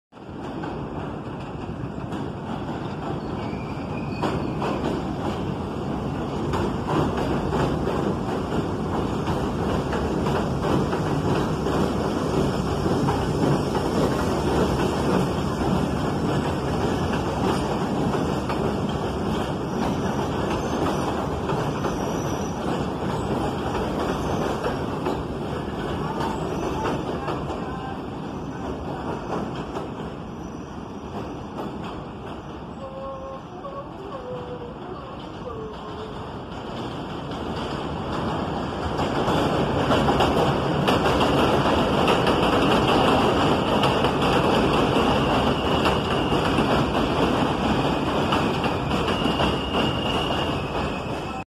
⁴ᴷ R32-R127 EP007 on Refuse Pickup 3 Passing Avenue U on the Culver Line
Here we see a R32 Refuse train passing Avenue U heading to Avenue X yard. Enjoy!